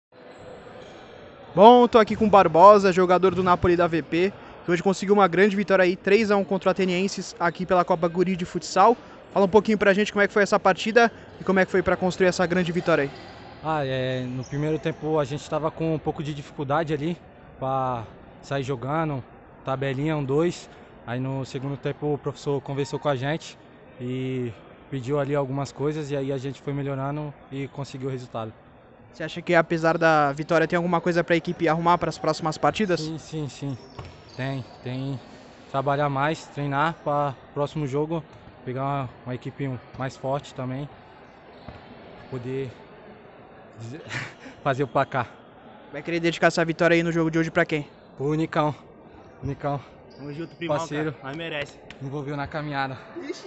Barbosa, Nápoli da VP
O jogador Barbosa, do Nápoli da VP, falou ao Passou por Dois após a partida contra o Atenienses, válida pela Copa Gury de Futsal.